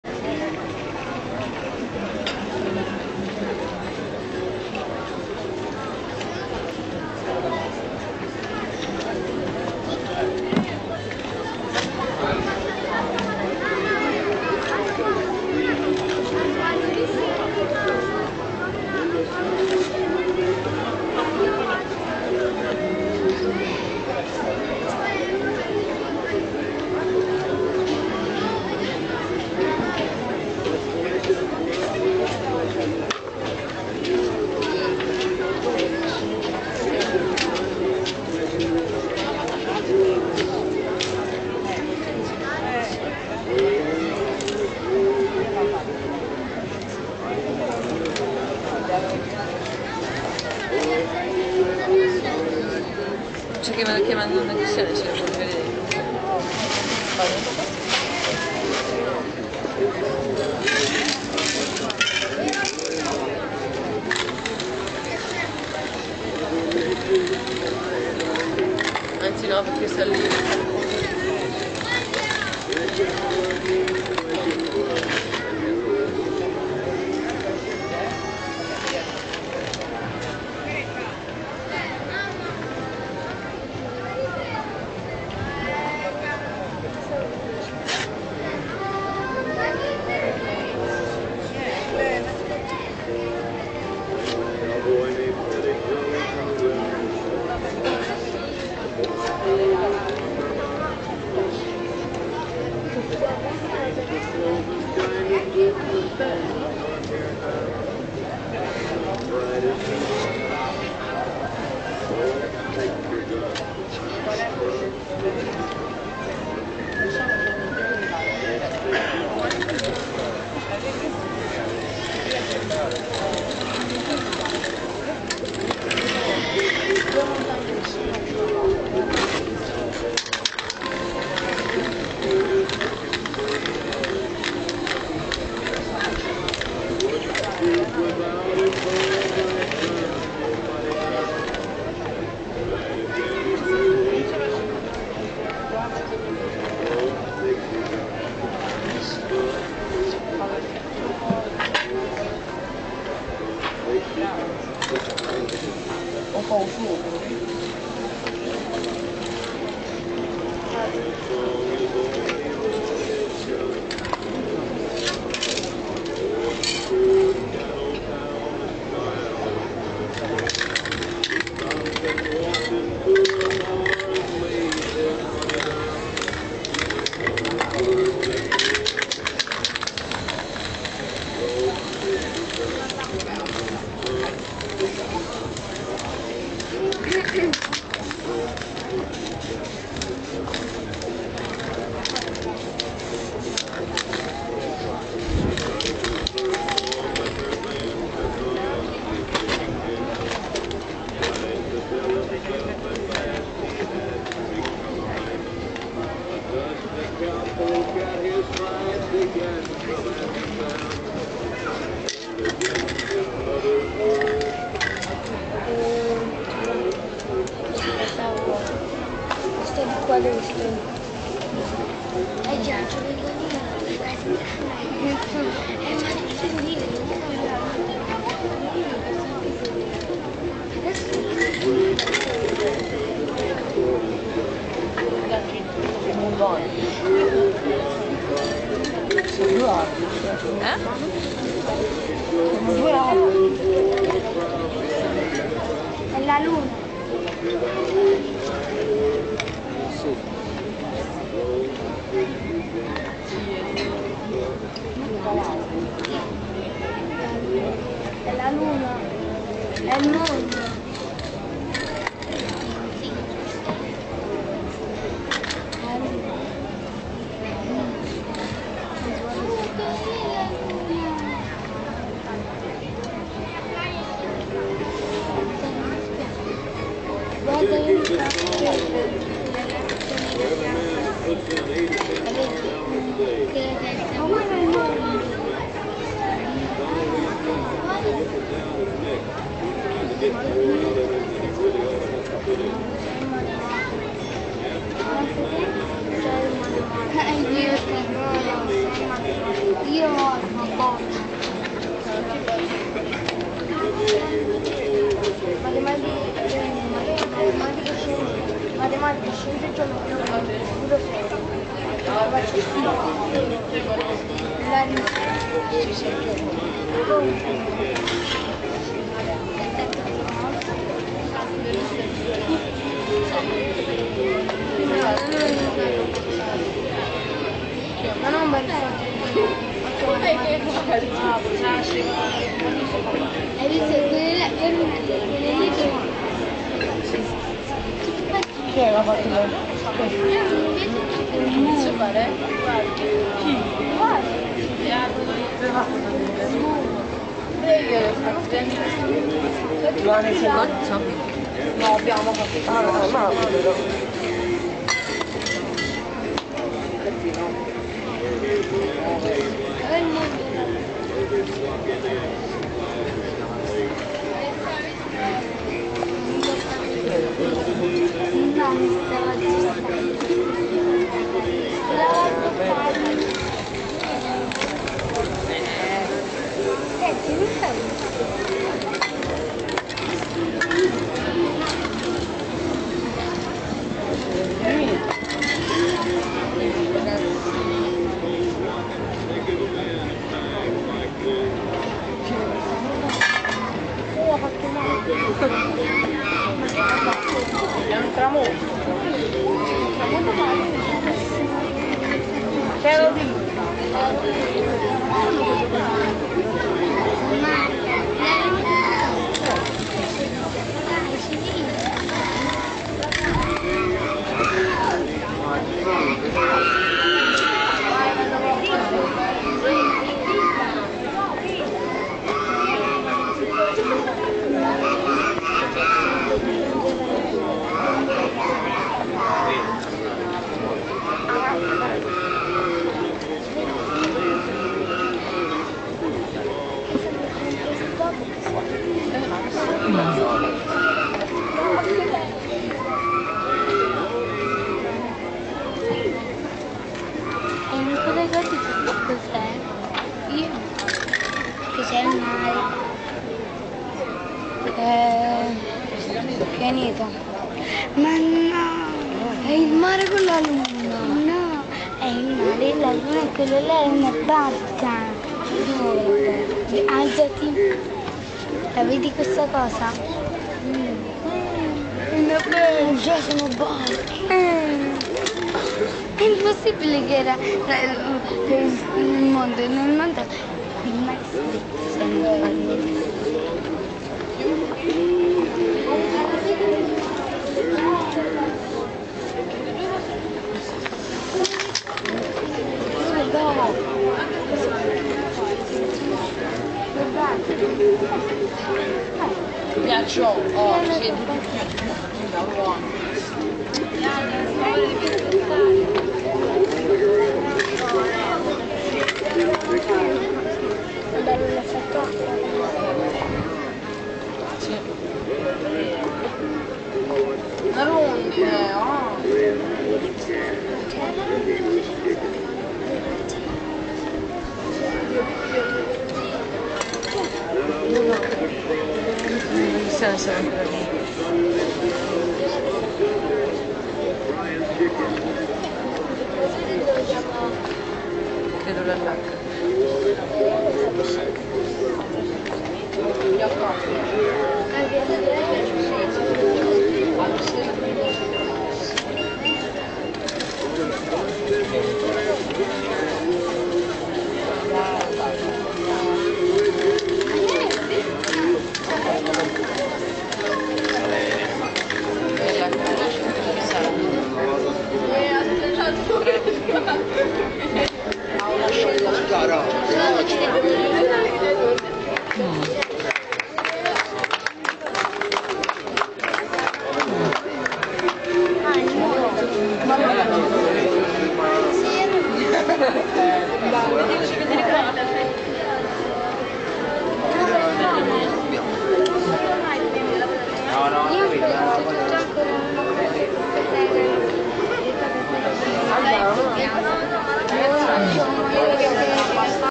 Street Spray Paint Art a Sperlonga by Mauro Sposito
Spray Paint Art by Mauro Sposito Sperlonga Italien augusti 2009
SprayArt Spray Paint Art World  (Mauro Sposito) 0055
Pittura allo spruzzo (spray)
La peinture a la bombe
Aereolsolgrafia
Sadotgrafia